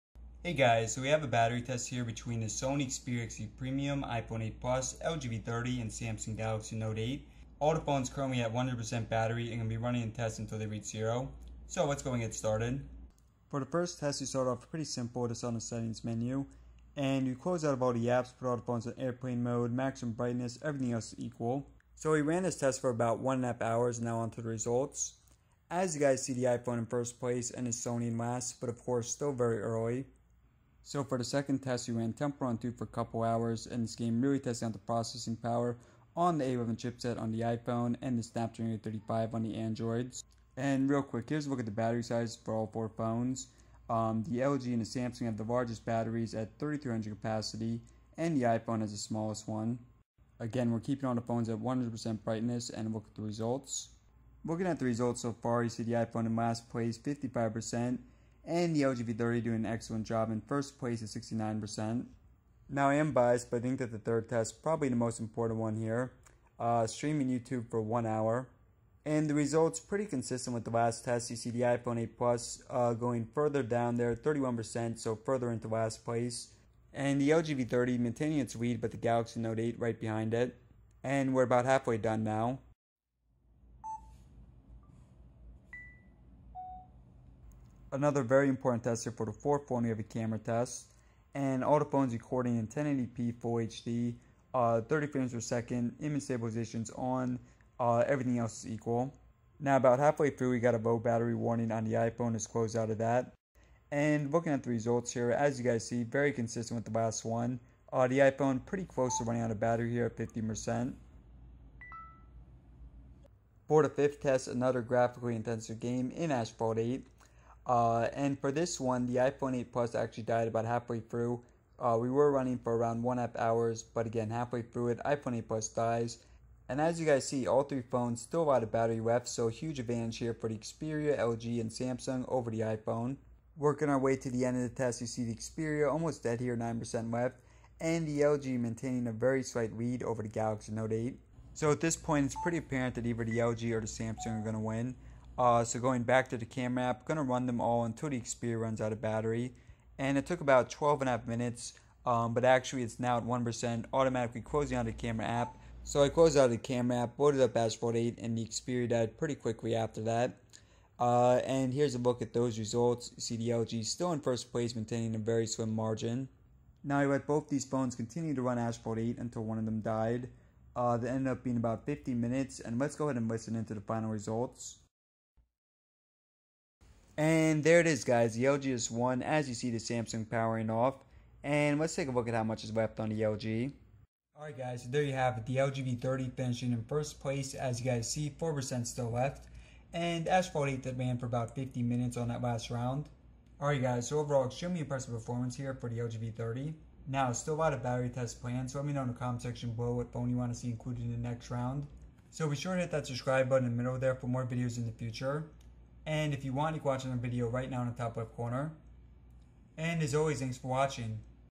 Battery Life Test! LG V30 vs Galaxy Note 8 vs iPhone 8 Plus vs Xperia XZ Premium
Battery Life Drain Test! LG V30 vs vs Samsung Galaxy Note 8 vs iPhone 8 Plus vs Sony Xperia XZ Premium. From 100%-0%.